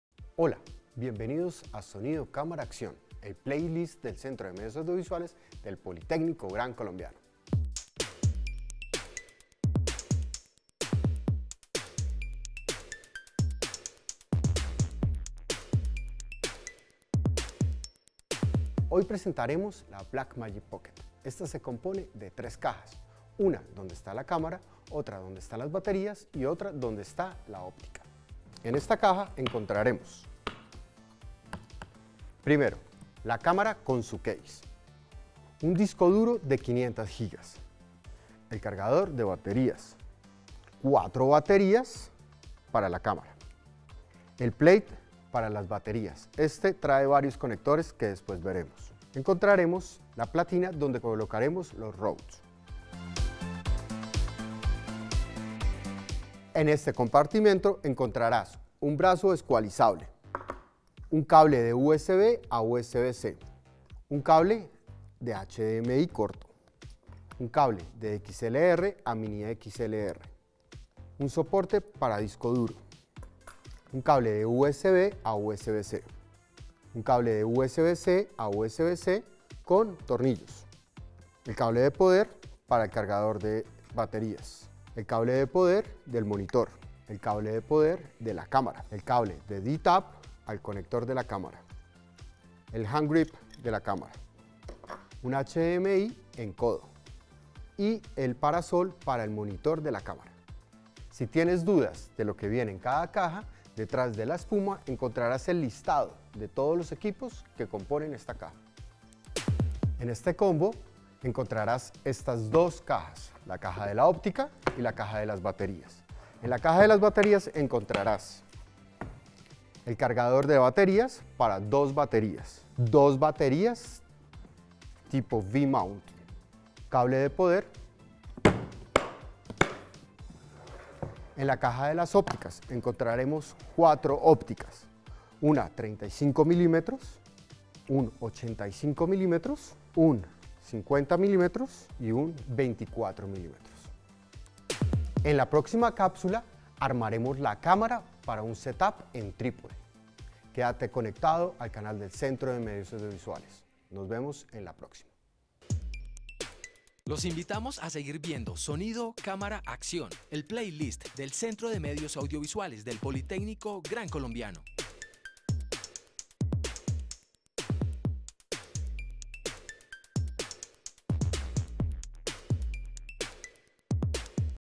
Unboxing: Black Magic Pocket en español
00:04 Presentación Andrés Pilonieta - Director Centro de Medios Audiovisuales
00:20 Presentación de la Black Magic Pocket - Unboxing
01:53 Descripción del combo de la Black Magic Pocket
01:57 Caja de las baterías
02:12 Caja de las ópticas - Descripción

Te presentamos la Black Magic Pocket Cinema Camara 4K, una cámara ideal para videos y fotografías en alta resolución. Descubre los accesorios y todo lo que incluye esta poderosa cámara con el CMA del Poli.